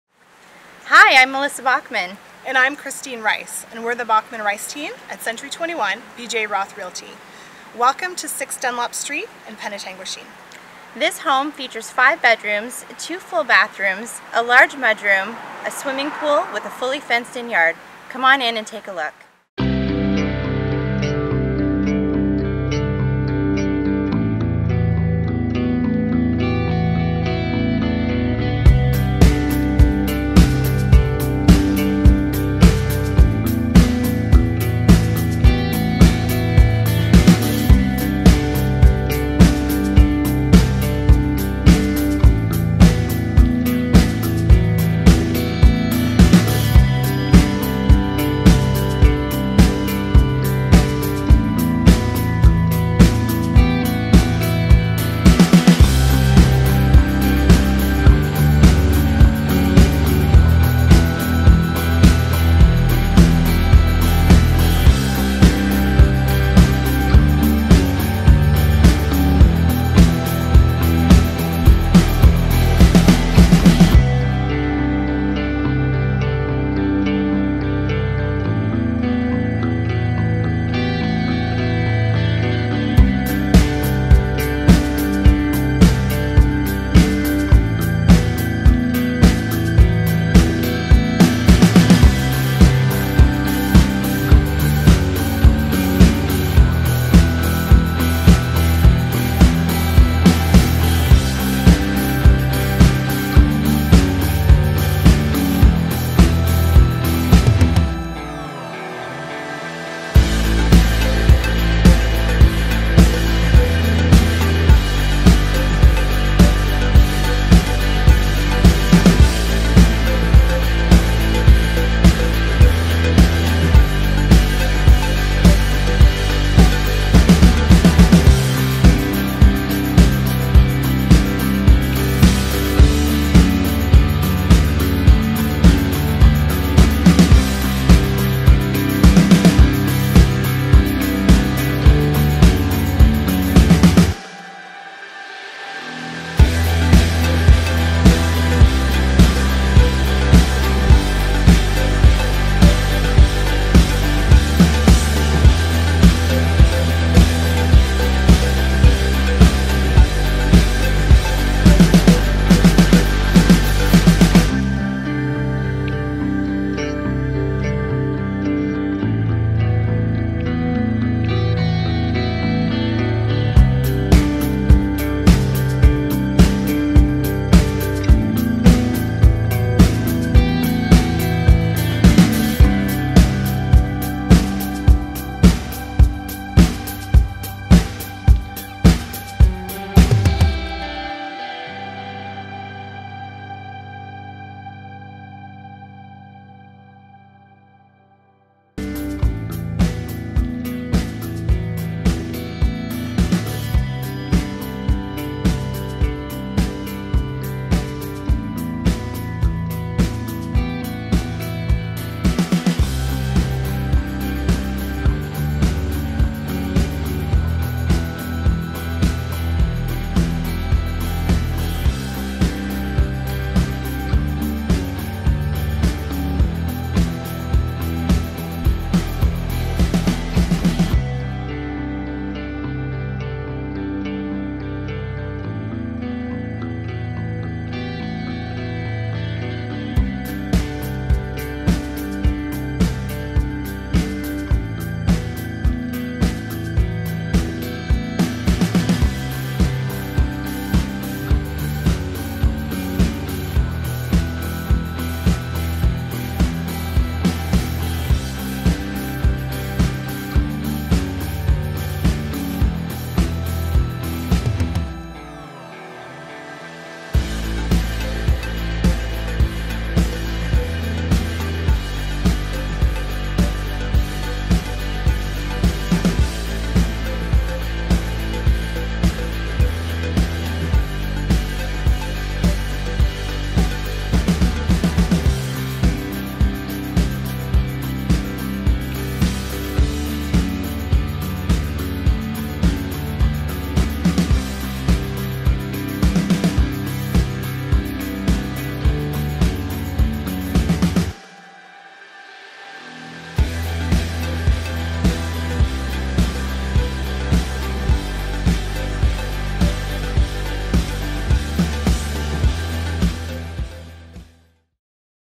6 Dunlop Street, Penetanguishene
Welcome to 6 Dunlop Street

This 5 bedroom, 2 bathroom home has a fully fenced yard, with a swimming pool, and attached garage with inside entry.

A great neighbourhood, close to the school, and ready for new owners.